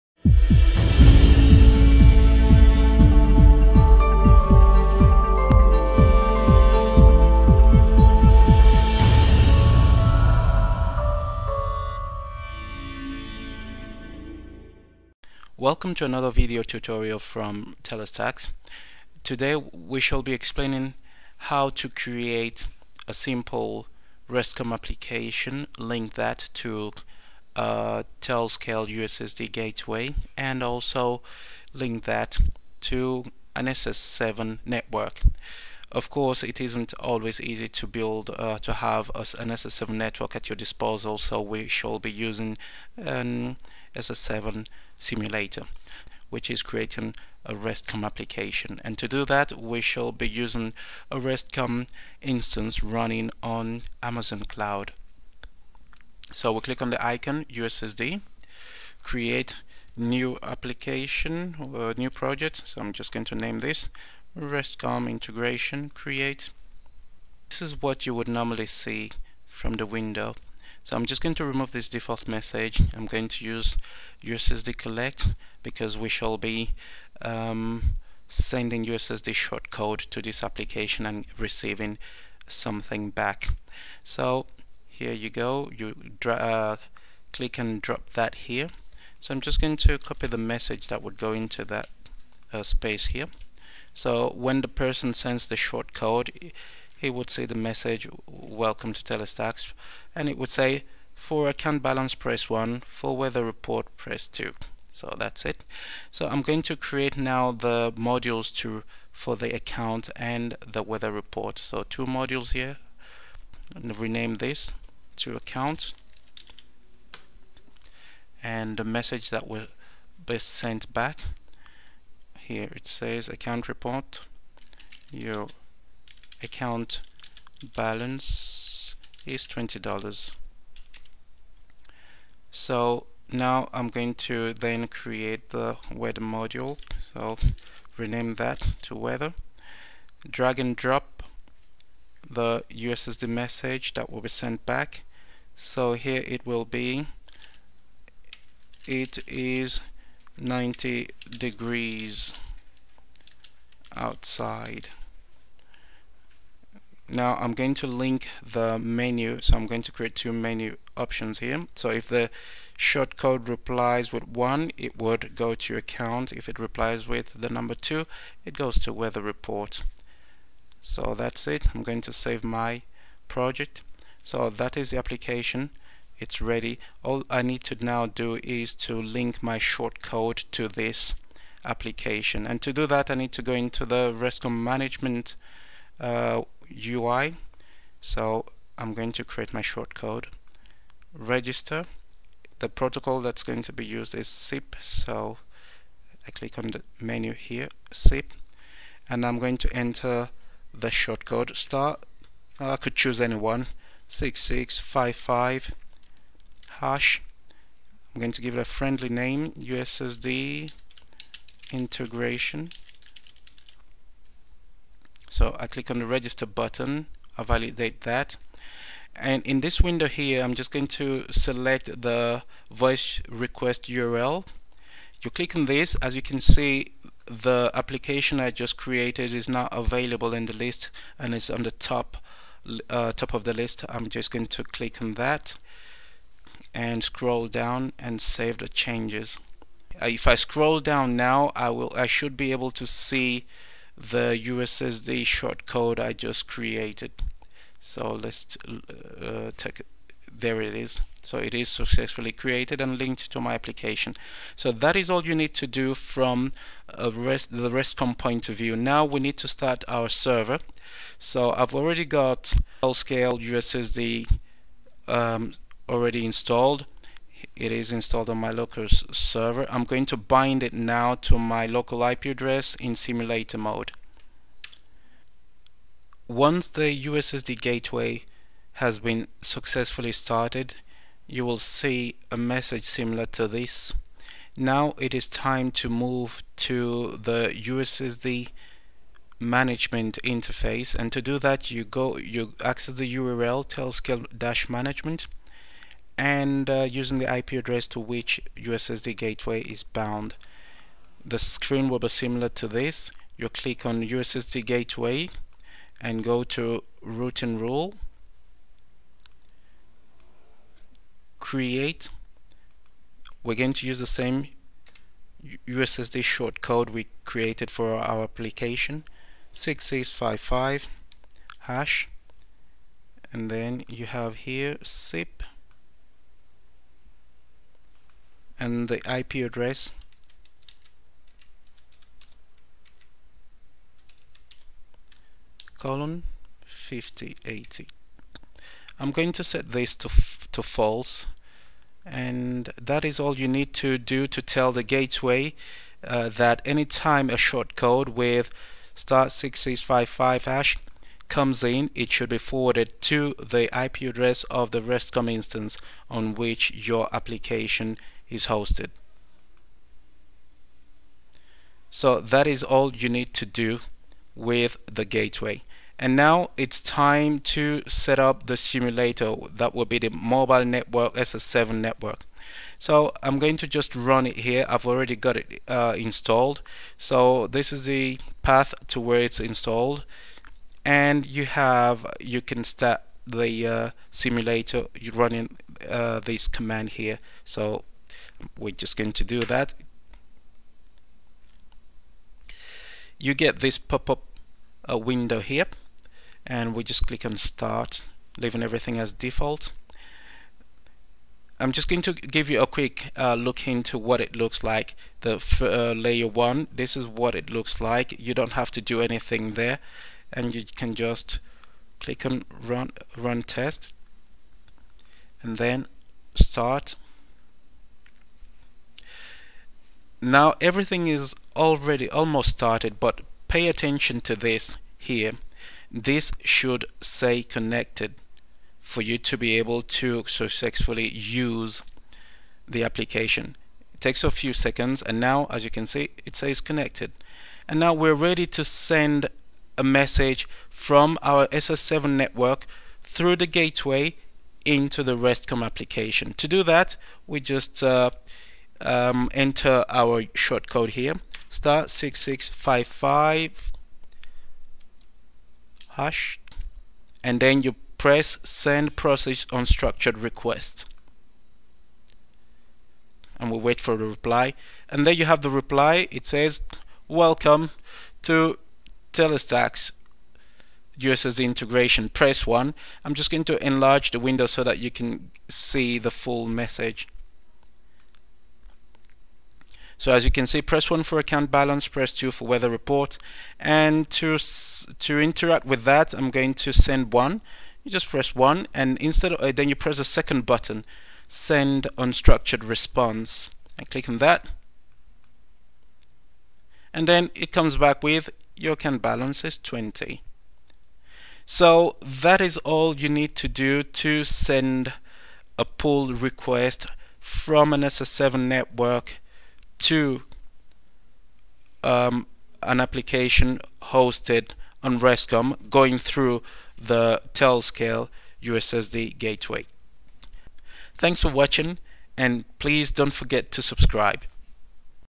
Telestax USSD Gateway with Restcomm Integration on a Mobile SS7 Simulator
Telestax USSD Restcomm Integration with Mobile SS7. This is a cool feature that shows how Telscale USSD Gateway can be integrated with Restcomm to build great applications.